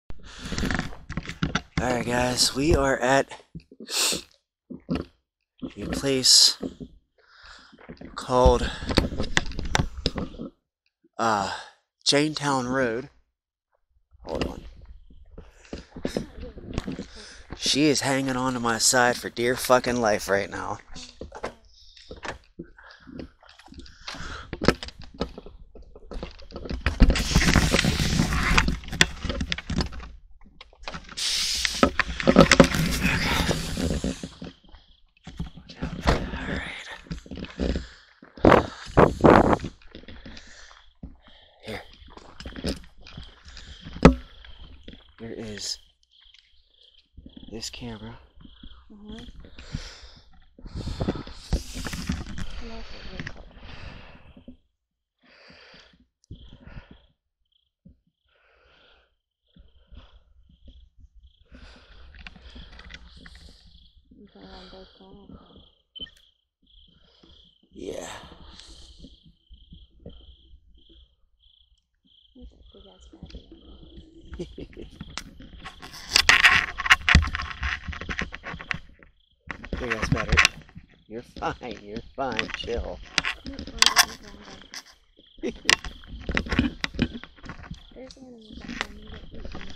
9/30/21 Chaintown Rd#1 Ashley was terrified
Ashley was scared so we didnt stay long at all. We went out to Chaintown Rd in Dawson pa. This road has a history of prisoners getting hung by the train bridge on this road long ago. Hints the name Chaintown road, They would take the chain gang out have them do the road work then after they was done they would hang them.